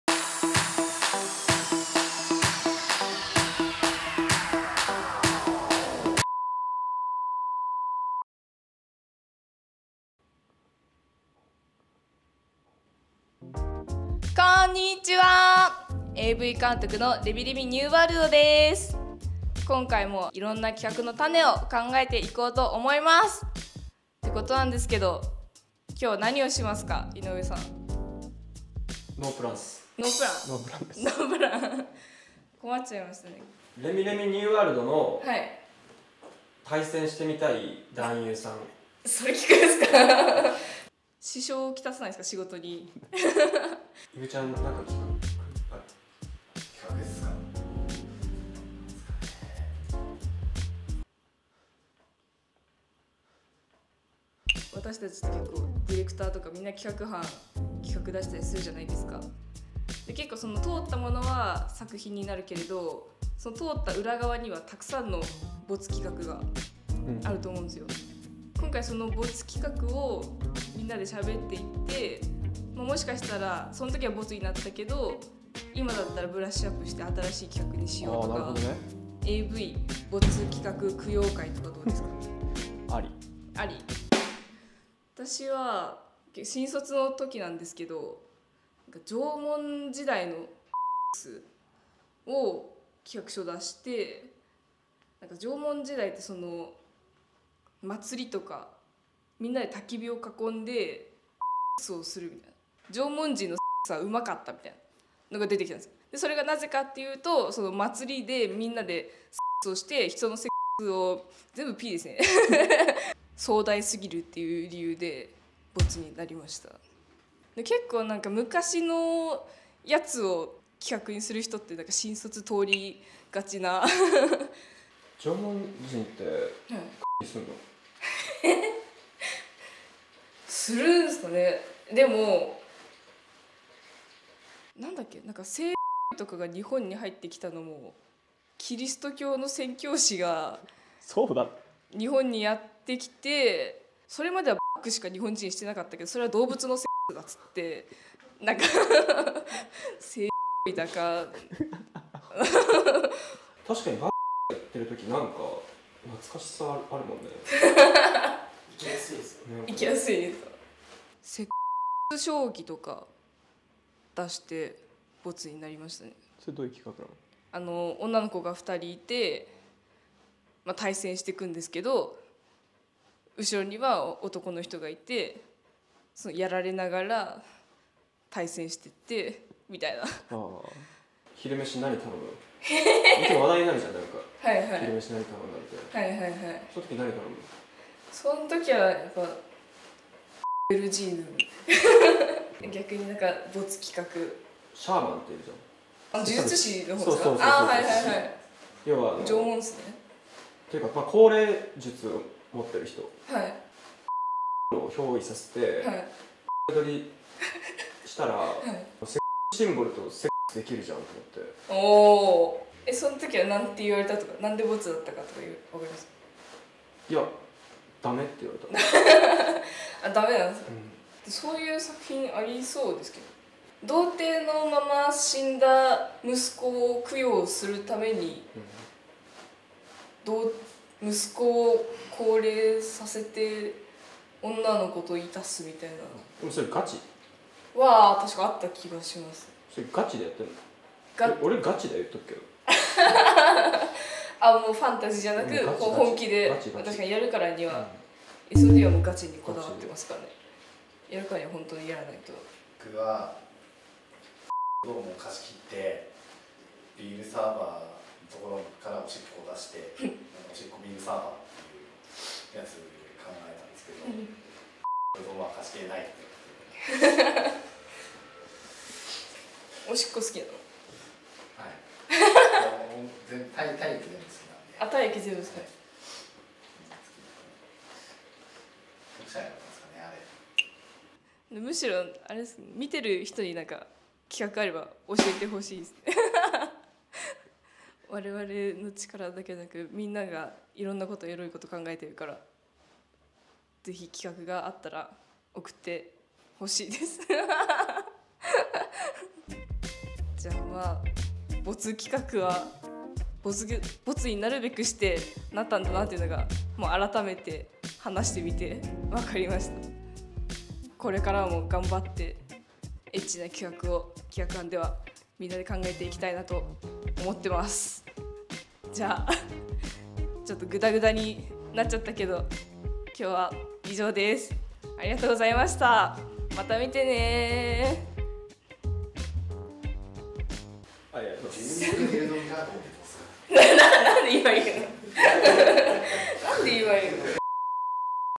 SOD企画班のいったんやってみよう。#1
SOD企画制作班が思い付きとノリだけで一旦撮影してみて、新たな作品のアイディアになればいいなぁ。（願望）という試みを始めちゃいます！